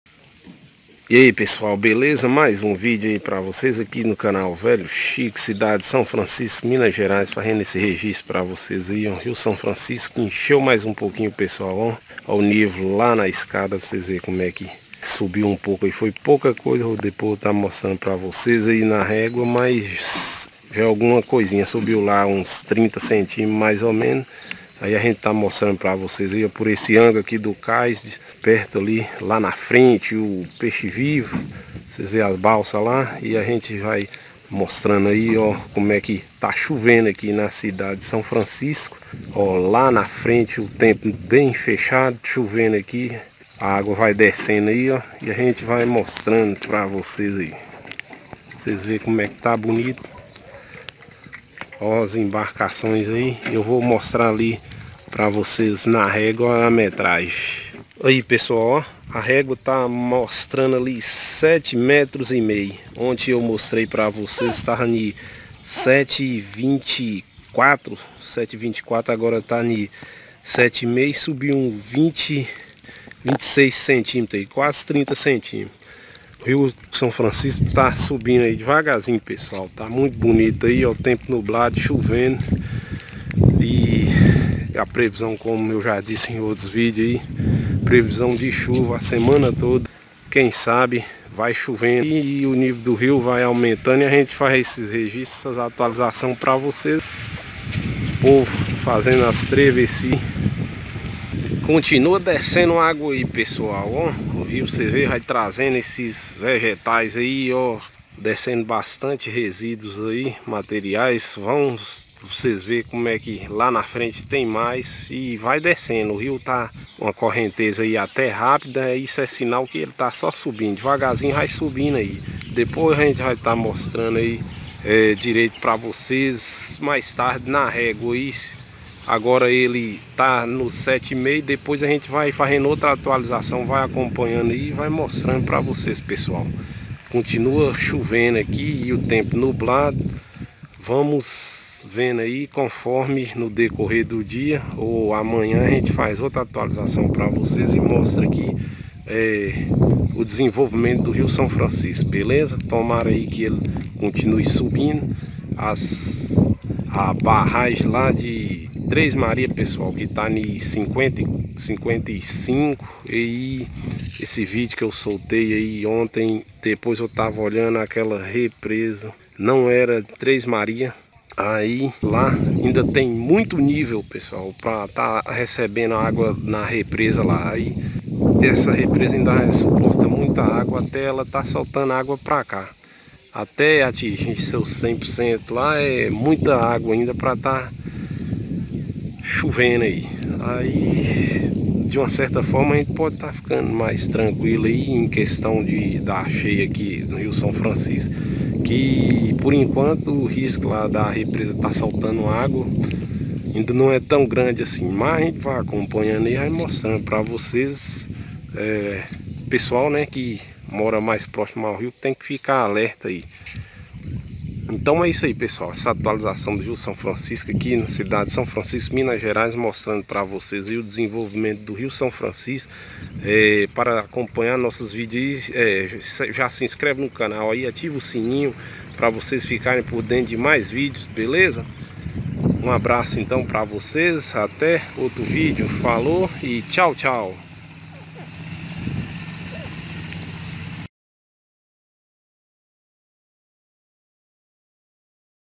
Rio São Francisco enchendo mais, atualização dia 4 de janeiro 2022
Velho Chico

CURTA
COMARTILHE
E SE-INSCREVA